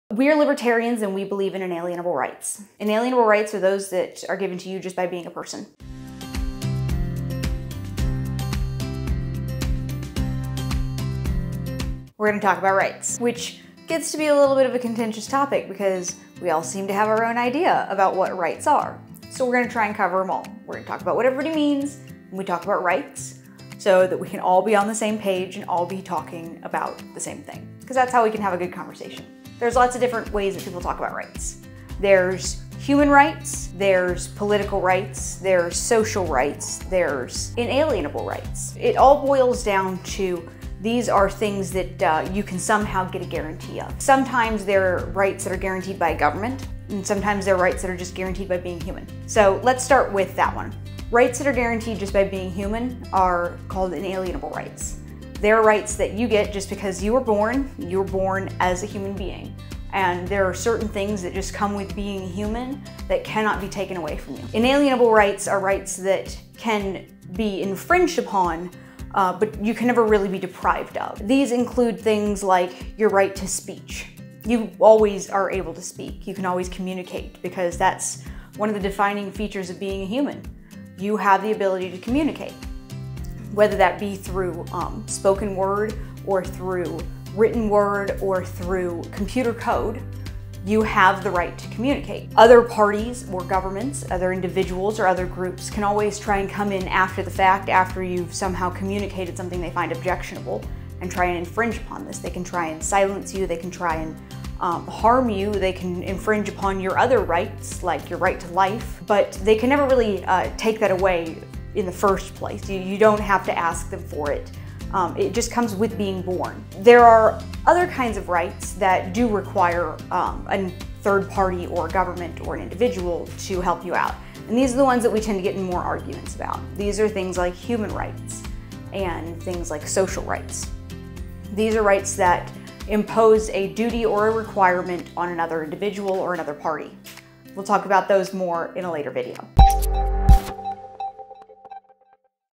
Fundamental Freedoms: What Are Rights?
Sarah Brady Wagner explains the different types of rights in our weekly video series Fundamental Freedoms.